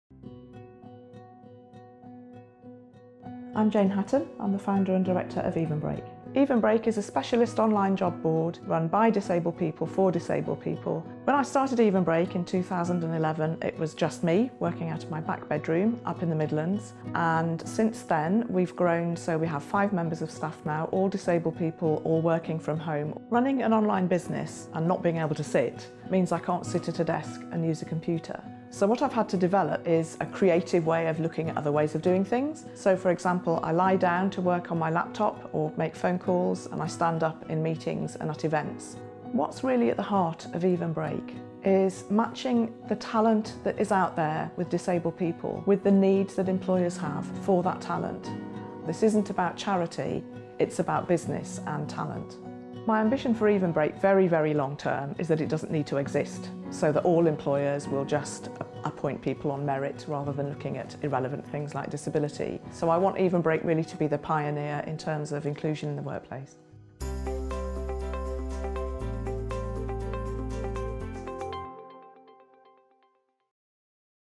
Jane Hatton from Evenbreak finalist of Stelios Awards for Disabled Entrepreneurs
Jane Hatton is the founder of Evenbreak an accessible specialist online job board where inclusive employers can attract more talented disabled talent and disabled jobseekers can find work with employers who will value their skills.